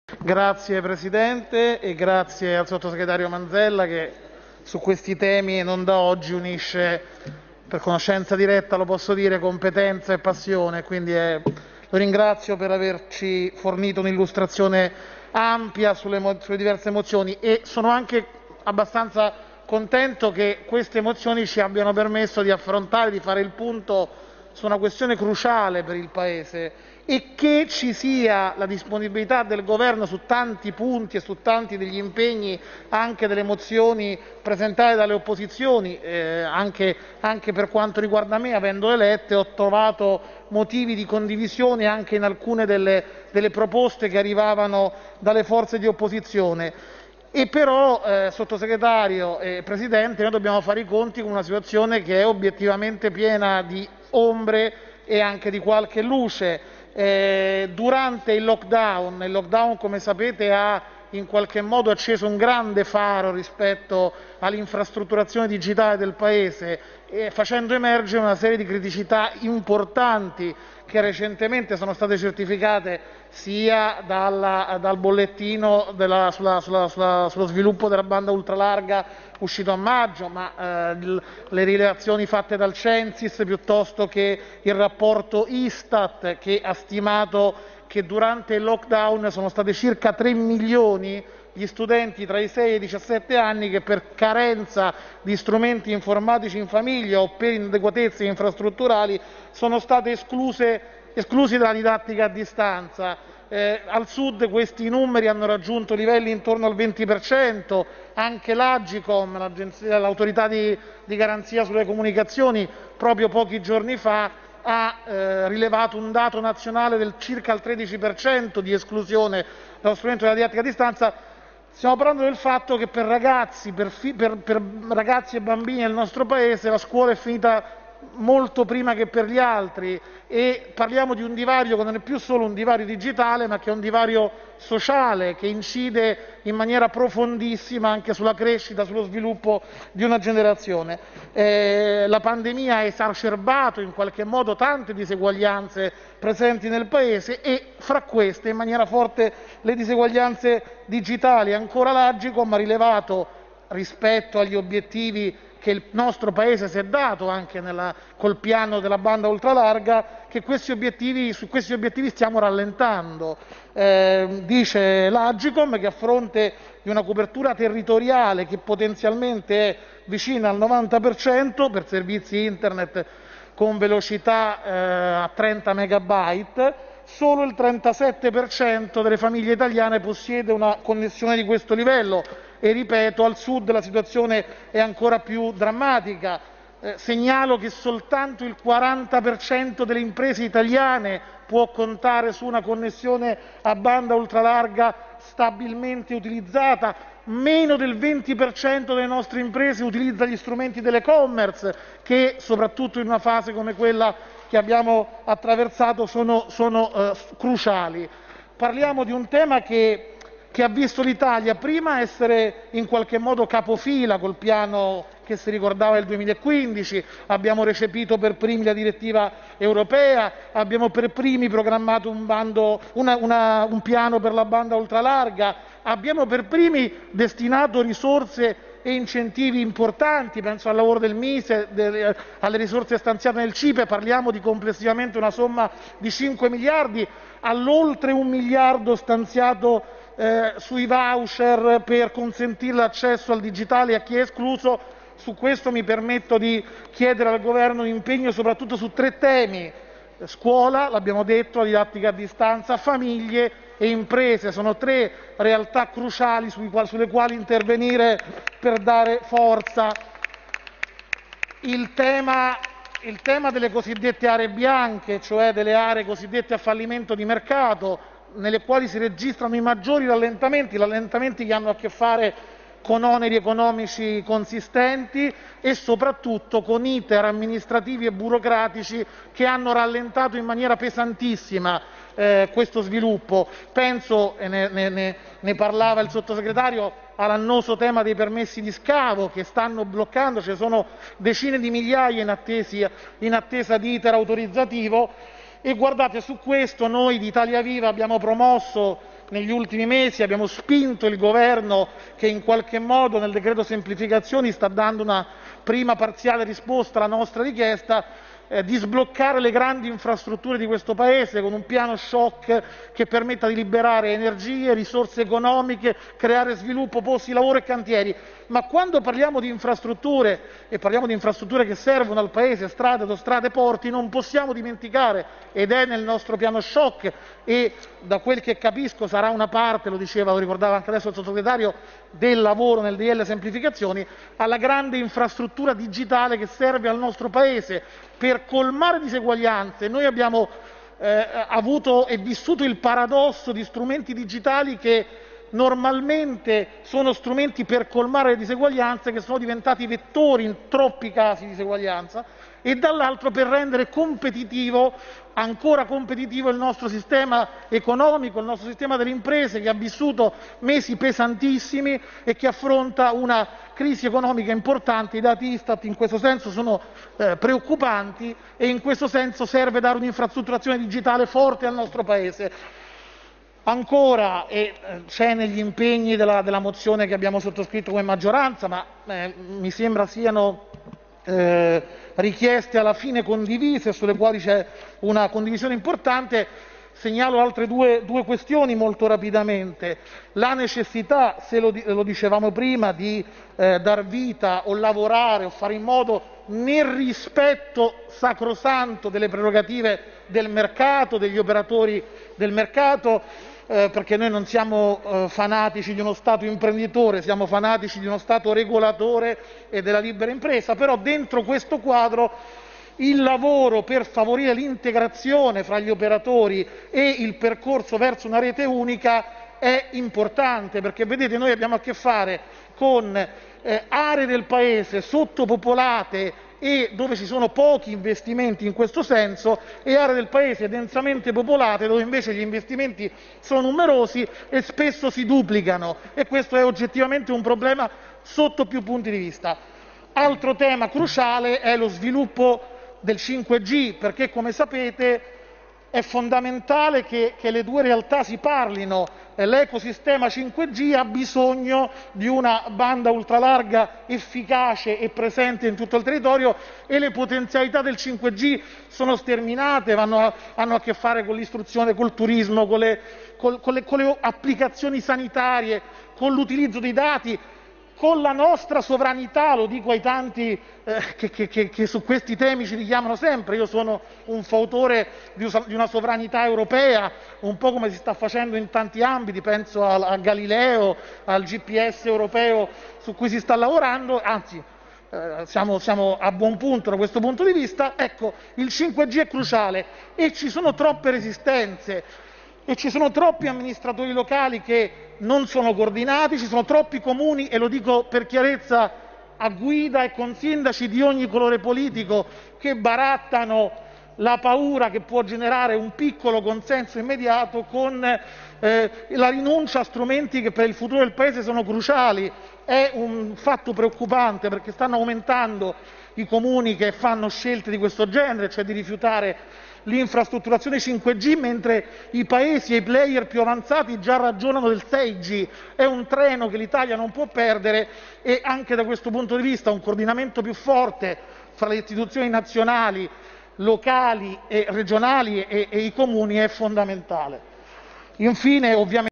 5G, banda ultra larga serve un svolta per le infrastrutture del Paese.
5G, banda ultra larga serve un svolta per le infrastrutture del Paese.

Oggi Luciano Nobili in dichiarazione di voto alla Camera dei Deputati